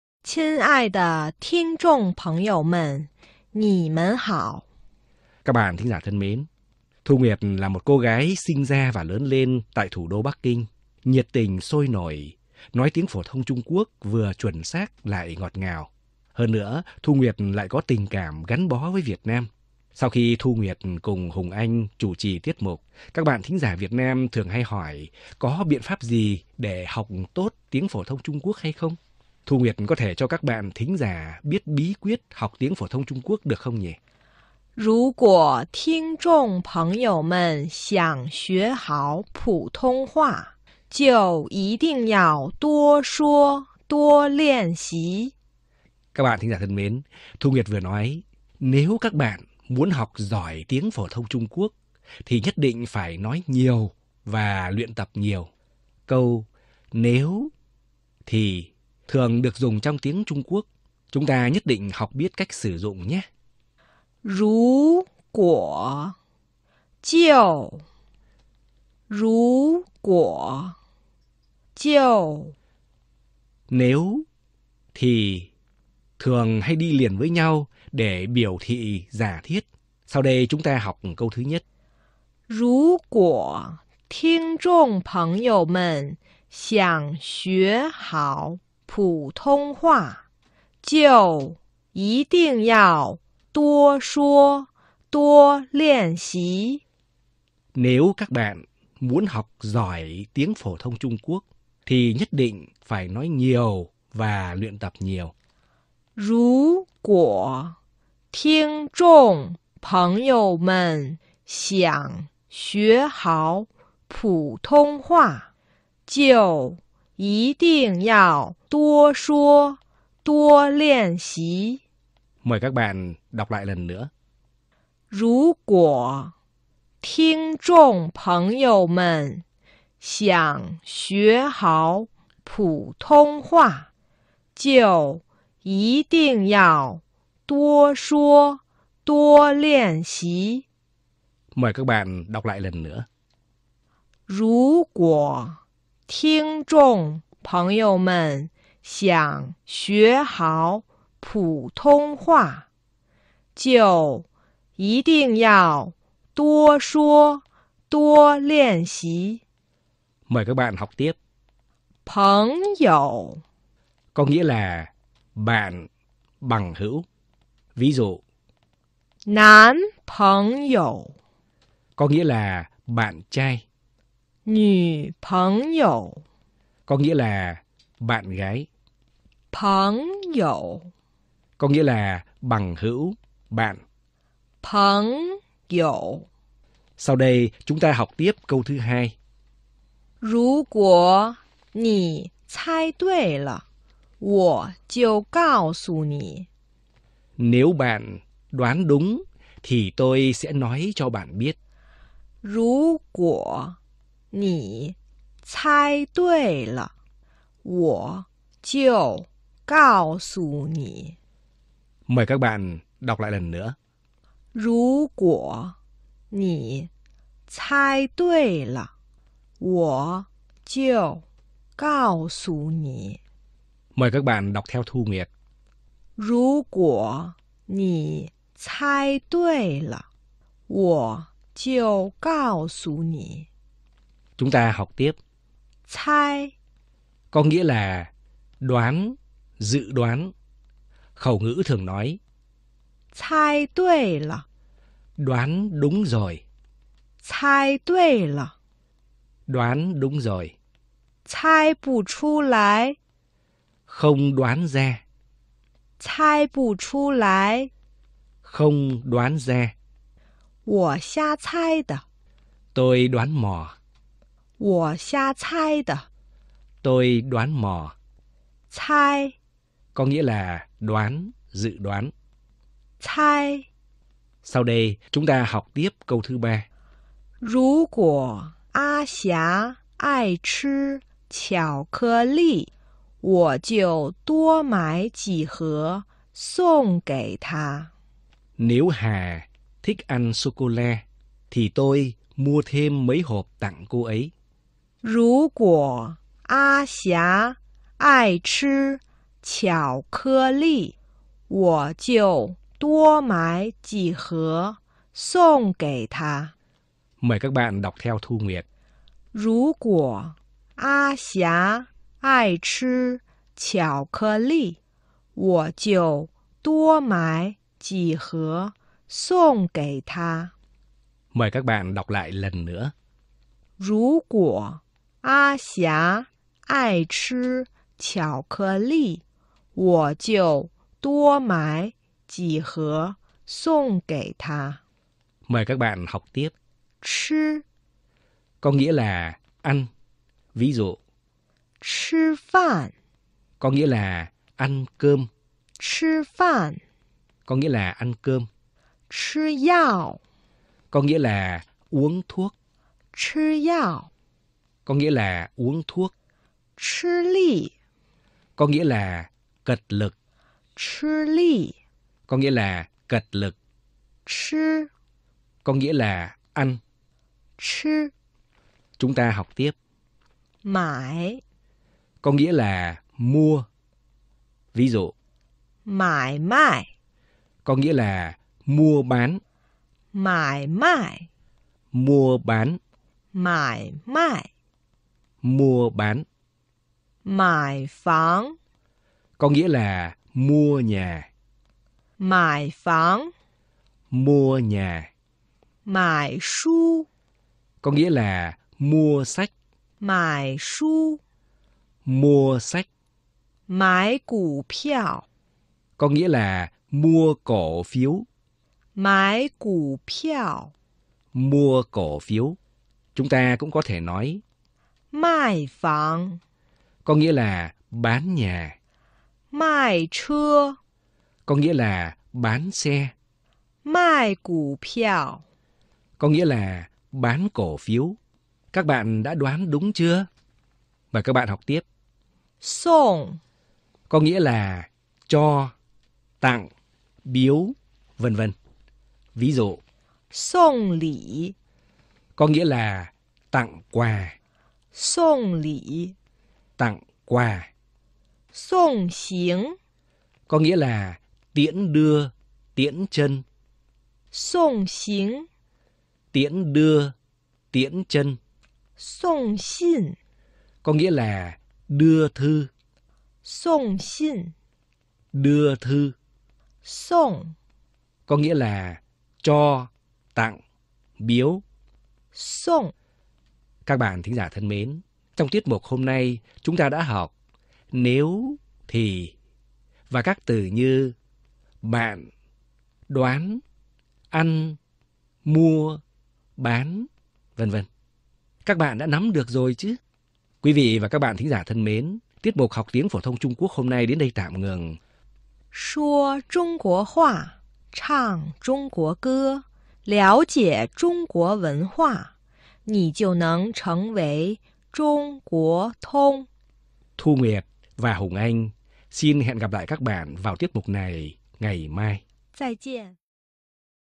Bài 2："如果……就……"句式
"如果……就……"， (rú ɡuǒ … … jiù … …)

Nếu... thì...thường hay đi liền với nhau để biểu thị giả thiết.

Sau đây chúng ta học câu thứ nhất:

(rú ɡuǒ tīnɡ zhònɡ pénɡ you men xiǎnɡ xué hǎo pǔ tōnɡ huà ， jiù yí dìnɡ yào duō shuō duō liàn xí.)

如果听众朋友们想学好普通话，就一定要多说多练习。

Nếu các bạn muốn học giỏi tiếng phổ thông Trung Quốc, thì nhất định phải nói nhiều và luyện tập nhiều.

朋友 (pénɡ you),

có nghĩa là bạn, bằng hữu.

Ví dụ :男朋友 (nán pénɡ you), có nghĩa là bạn trai.

女朋友 (nǚ pénɡ you) , có nghĩa là bạn gái.

Sau đây chúng ta học tiếp câu thứ hai :

(rú ɡuǒ nǐ cāi duì le ， wǒ jiù ɡào su nǐ.)

如果你猜对了，我就告诉你；

Nếu bạn đoán đúng thì tôi sẽ nói cho bạn biết.

猜 (cāi) ,

có nghĩa là đoán, dự đoán.

Khẩu ngữ thường nói:

猜对了（cāi duì le）, Đoán đúng rồi,

猜不出来 （cāi bù chū lái）, Không đoán ra,

我瞎猜的 （wǒ xiā cāi de）, Tôi đoán mò.

Sau đây chúng ta học tiếp câu thứ ba:

(rú ɡuǒ ā xiá ài chī qiǎo kè lì, wǒ jiù duō mǎi jǐ hé sònɡ ɡěi tā.)

如果阿霞爱吃巧克力，我就多买几盒送给她。

Nếu Hà thích ăn sô-cô-la, thì tôi mua thêm mấy hộp tặng cô ấy.

吃（chī），

có nghĩa là ăn.

Ví dụ : 吃饭（chī fàn）， có nghĩa là ăn cơm，

吃药（chī yào）， có nghĩa là uống thuốc，

吃力 （chī lì），có nghĩa là cật lực.

买 (mǎi) ,

có nghĩa là mua.

Ví dụ : 买卖(mǎi mɑi), có nghĩa là mua bán

买房(mǎi fánɡ), có nghĩa là mua nhà

买书 (mǎi shū), có nghĩa là mua sách

买股票 (mǎi ɡǔ piào) , có nghĩalàmua cổ phiếu

Chúng ta cũng có thể nói: 卖房 (mài fánɡ), có nghĩa là bán nhà，卖车(mài chē ), có nghĩa là bán xe，卖股票(mài ɡǔ piào) , có nghĩa là bán cổ phiếu. Các bạn đã đoán đúng chưa ?

送(sònɡ),

có nghĩa là cho, tặng, biếu.v.v...

Ví dụ :送礼 (sònɡ lǐ),có nghĩa là tặng quà

送行(sònɡ xínɡ),có nghĩa là tiễn đưa, tiễn chân

送信(sònɡ xìn), có nghĩa là đưa thư